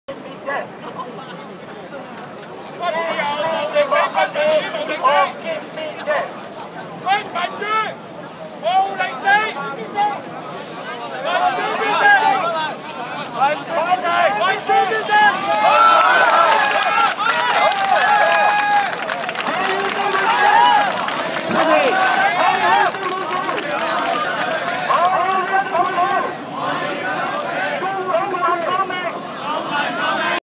金钟清场 - 李柱铭大律师被拘捕
12月11号从上午9点左右开始，学联和学民思潮的部分成员在政府总部外的马路上静坐。随后，民主党创党主席李柱铭大律师、壹传媒主席、苹果日报老板黎智英、民主党议员、支联会主席何俊仁律师、公民党党魁梁家杰议员、民主党主席刘慧卿议员，以及包括艺人何韵诗在内的大批支持者也来到该处静坐，等候警方在清场时拘捕。

香港警方12月11号晚上9点左右将学联秘书长周永康和李卓人议员最后带走，完成全部约150位静坐留守者的拘捕。脸书: